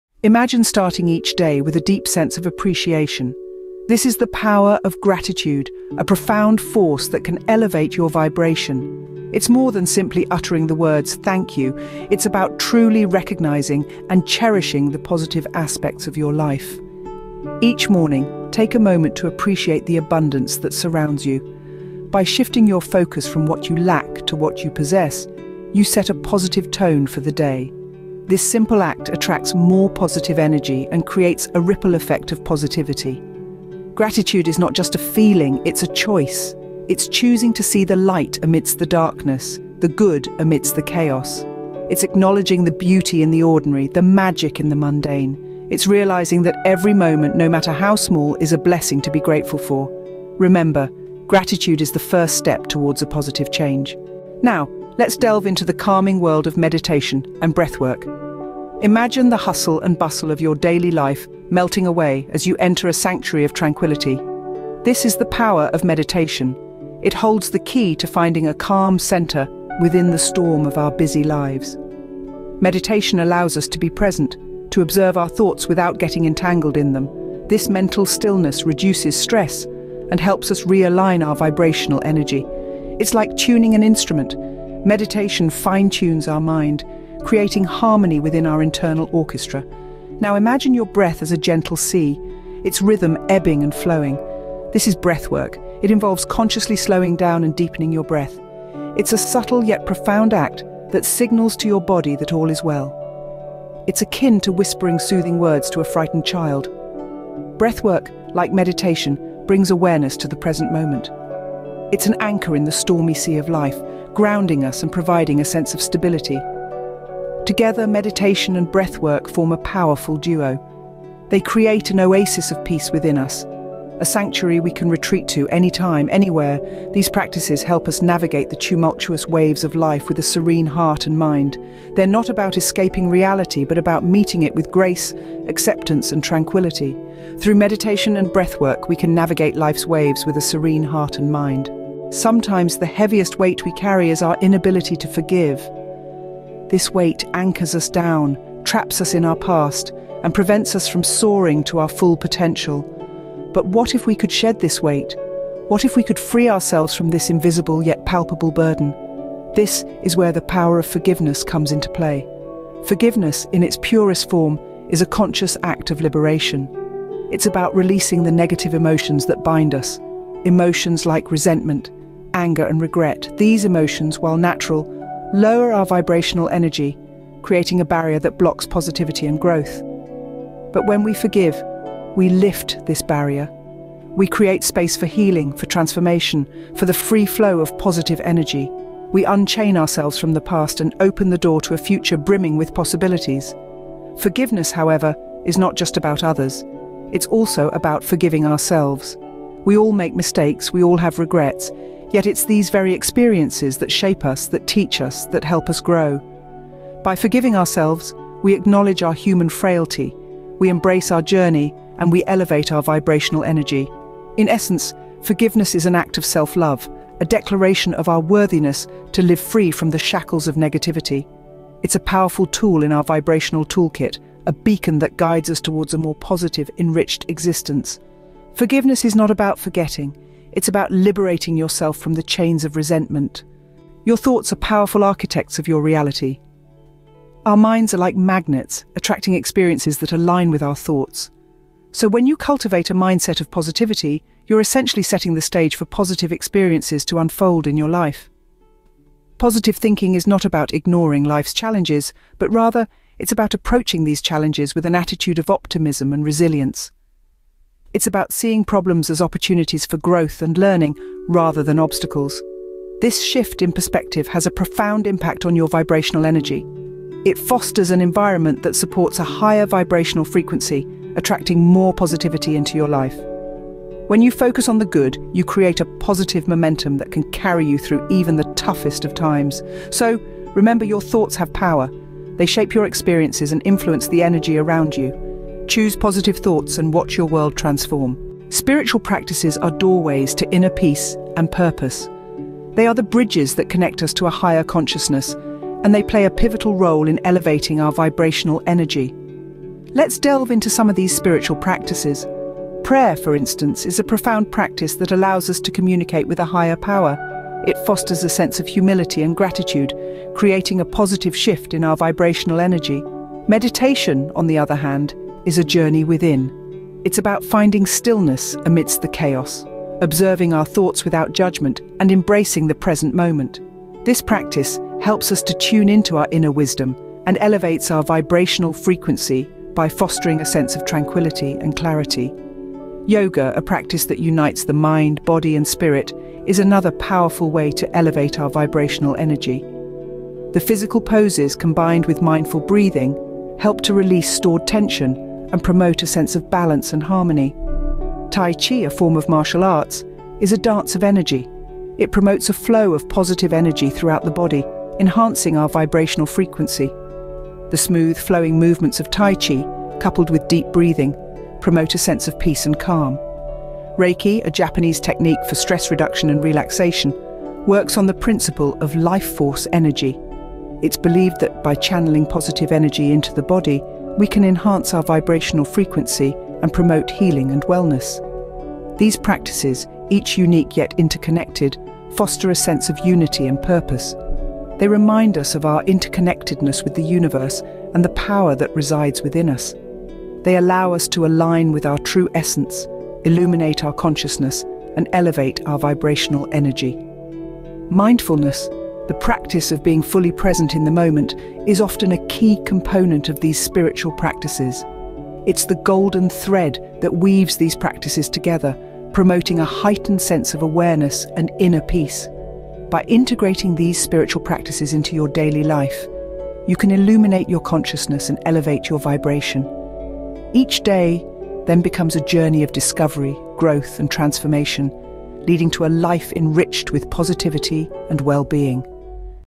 Elevate Your Vibration: Simple, Practical Techniques
Discover how to elevate your vibration and transform your energy with simple, practical techniques. In this enlightening video, we delve into the power of gratitude, meditation's tranquility, forgiveness's release, positive thinking's impact, and enriching spiritual practices. Whether you're new to energy work or looking to deepen your spiritual journey, these strategies are designed to boost your vibrational frequency and enhance your overall well-being.

Video Outline:
Awakening Gratitude: Learn how embracing gratitude can shift your mindset and elevate your vibration.
The Power of Meditation and Breathwork: Explore meditation and breathwork techniques to find inner peace and clarity.
The Liberation of Forgiveness: Understand the transformative power of forgiveness and how it can free your energy.
The Influence of Positive Thinking: Discover the impact of positive thinking on your energy and overall outlook on life.
The Unity of Spiritual Practices: Delve into various spiritual practices that can unify and elevate your vibrational frequency.

Boost Your Vibration: Learn effective techniques to raise your vibrational frequency and improve your energy.
Enhance Well-Being: Discover practices that contribute to overall well-being and spiritual growth.
Practical Guidance: Get practical tips and techniques that you can easily incorporate into your daily life.